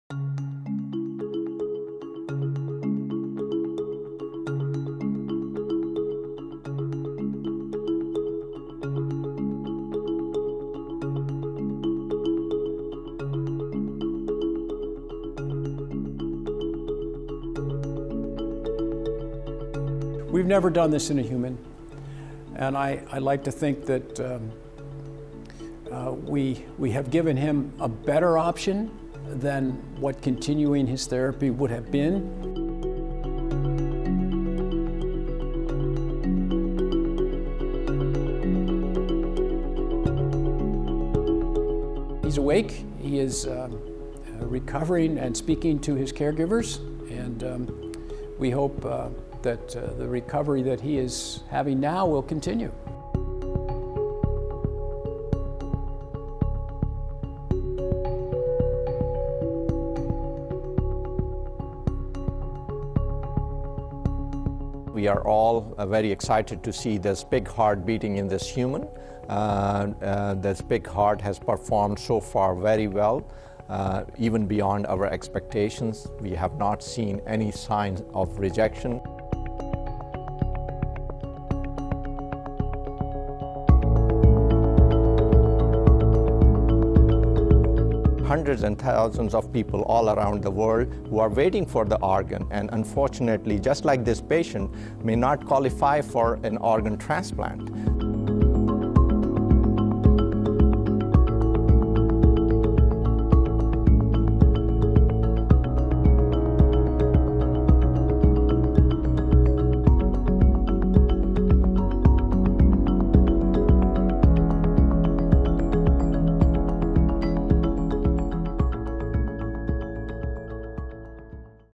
В США врачи впервые успешно пересадили человеку сердце свиньи
Хирурги в США провели первую в мире операцию, в ходе которой пациенту пересадили генетически модифицированное сердце свиньи. Врачи говорят, что для 57-летнего Дэвида Беннета — это был единственный шанс выжить. Сейчас мужчина чувствует себя хорошо.